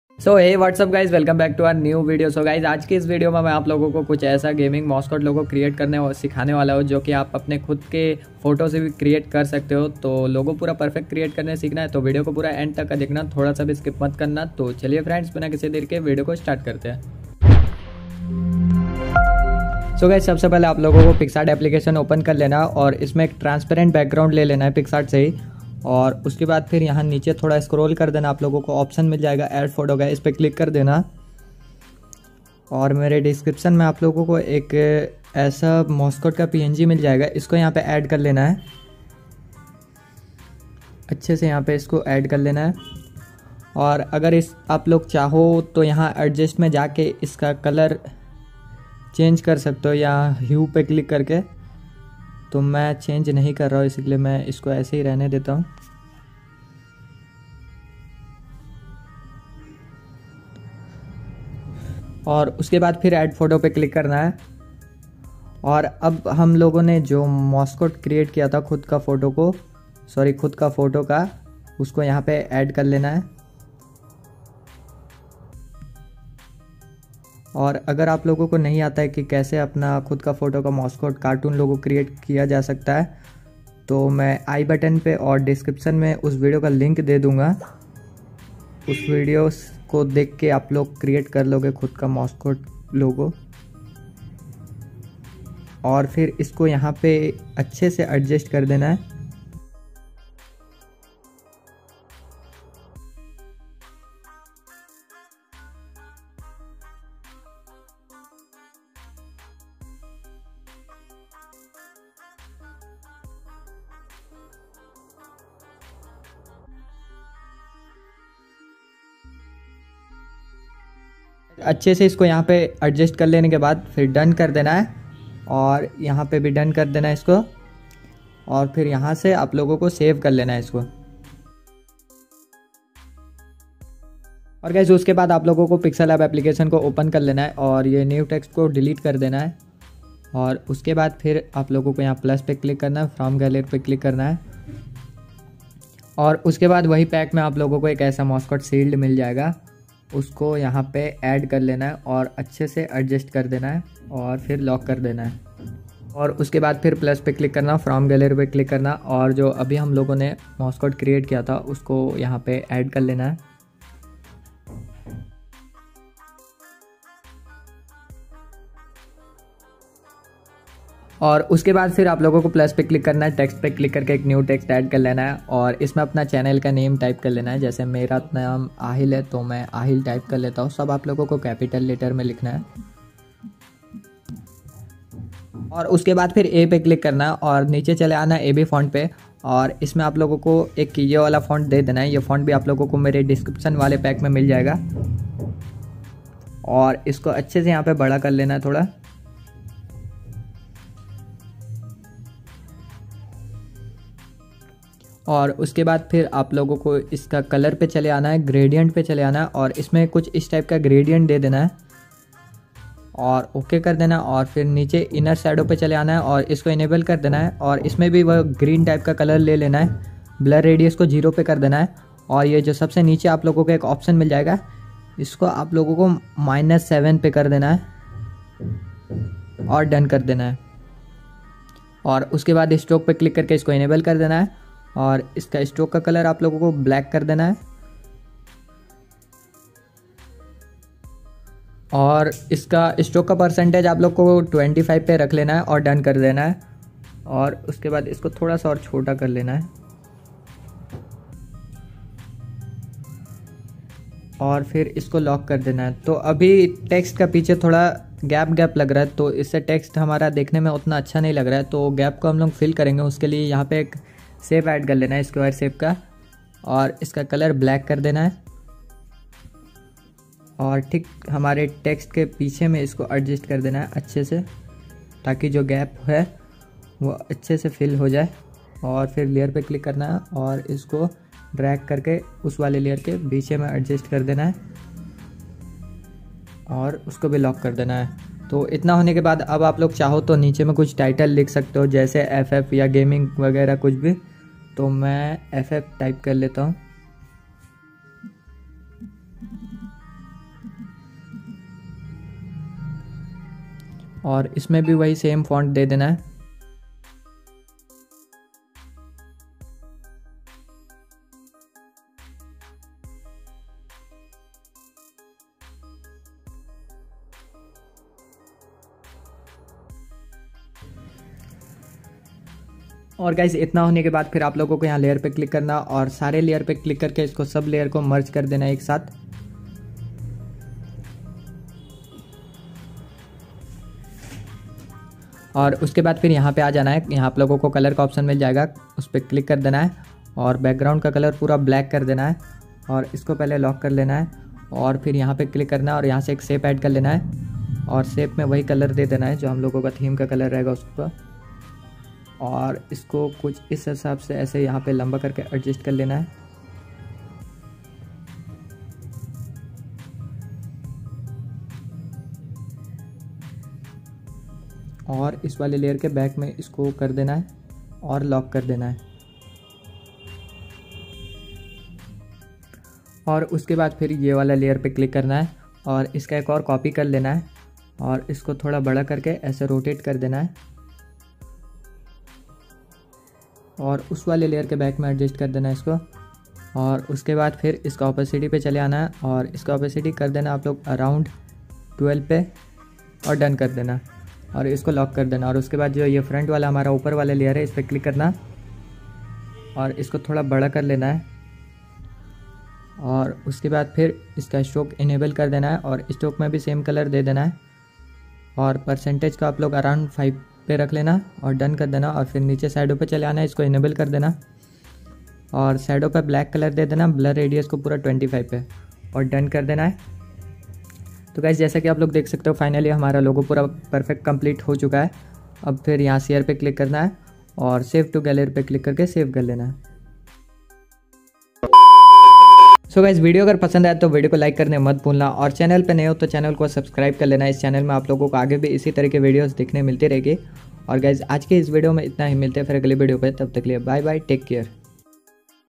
How To Make Your Own Photo Mascot Logo | On Android 100% Real 🤯
TOPIC
So, Guys Aaj Ki Is Video Me Mene Aapko Sikhiya Ki Aap Apne Mobile Se Ek Accha (INTRO) Kaise Bana Sakte ho Verry Easy Step, By Step, In Hindi..

Usefull Videos🔥